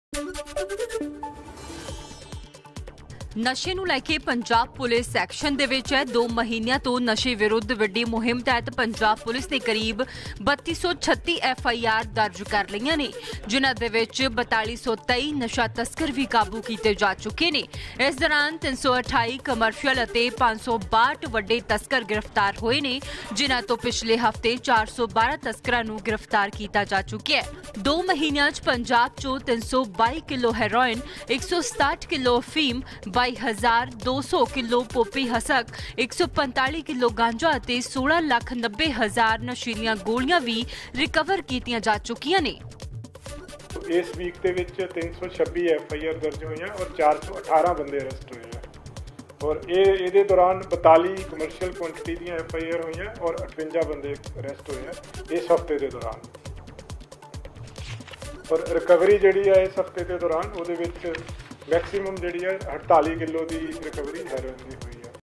ਨਸ਼ੇ ਨੂੰ ਲੈ ਕੇ ਐਕਸ਼ਨ 'ਚ Punjab Police, 4223 ਤਸਕਰ ਕਾਬੂ
ਨਸ਼ੇ ਨੂੰ ਲੈ ਕੇ ਪੰਜਾਬ ਪੁਲਿਸ ਸਰਗਰਮ ਐ ..2 ਮਹੀਨਿਆਂ ਤੋਂ ਨਸ਼ੇ ਵਿਰੁੱਧ ਵਿੱਢੀ ਮੁਹਿੰਮ ਤਹਿਤ ਪੰਜਾਬ ਪੁਲਿਸ ਨੇ ਕਰੀਬ 3236 FIR ਦਰਜ ਕੀਤੀਆਂ ਨੇ ਜਿਨ੍ਹਾ 'ਚ 4223 ਨਸ਼ਾ ਤਸਕਰ ਕਾਬੂ ਕੀਤੇ ਗਏ ਨੇ ...ਇਸ ਦੌਰਾਾਨ 328 ਕਮਰਸ਼ੀਅਲ ਤੇ 562 ਵੱਡੇ ਤਸਕਰ ਗ੍ਰਿਫ਼ਤਾਰ ਹੋਏ ਨੇ ਜਿਨ੍ਹਾੰ 'ਚੋਂ ਪਿਛਲੇ ਹਫ਼ਤੇ 412 ਤਸਕਰਾਂ ਨੂੰ ਗ੍ਰਿਫ਼ਤਾਰ ਕੀਤਾ ਗਿਆ ,. 2 ਮਹਿਨ੍ਆਂ 'ਚ ਪੰਜਾਬ 'ਚੋਂ 322 ਕਿਲੋ ਹੈਰੋਇਨ , 167 ਕਿਲੋ ਅਫ਼ੀਮ ,22,200 ਕਿਲੋ ਪੋਪੀ ਹਸਕ ,145 ਕਿਲੋ ਗਾਂਜਾ  ਤੇ 16 ਲੱਖ 90 ਨਸ਼ੀਲੀਆਂ ਗੋਲਈਆਂ ਰਿਕਵਰ ਹੋਈਆਂ ਨੇ .